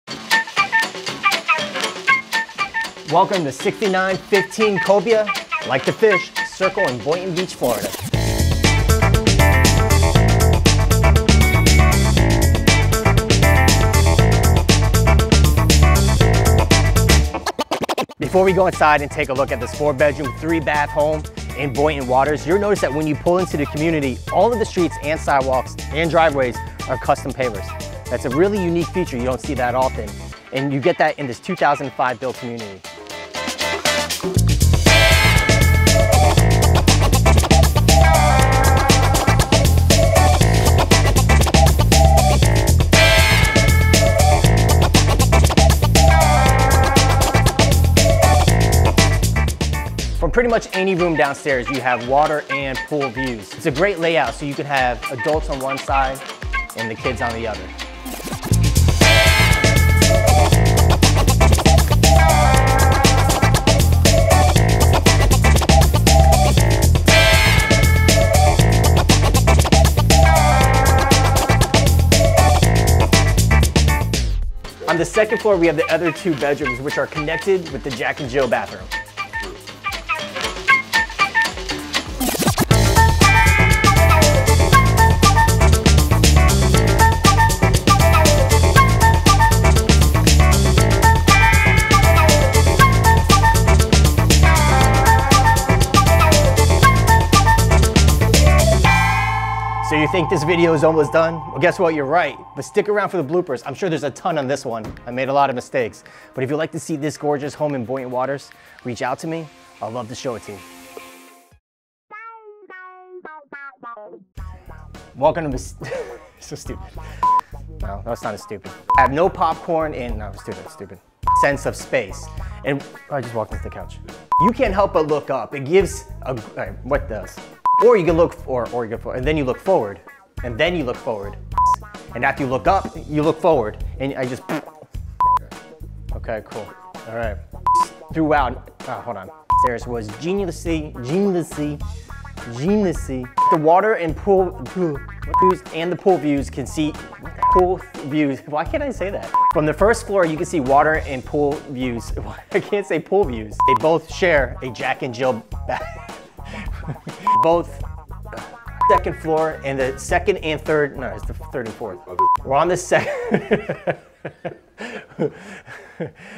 Video Home Tour | 6915 Cobia Cir Boynton Beach, FL | Bloopers at The End
🤨Out-takes at 1:52🤨
🚨 NEW HOME OF THE WEEK 🚨
🏡Boynton Waters🌴
💰 $500,000
🛌 4 Beds | 3 Baths | 3,364 SF 💠
💧 Screened-in Pool | Lake Lot
⬇️ Super Low HOA Dues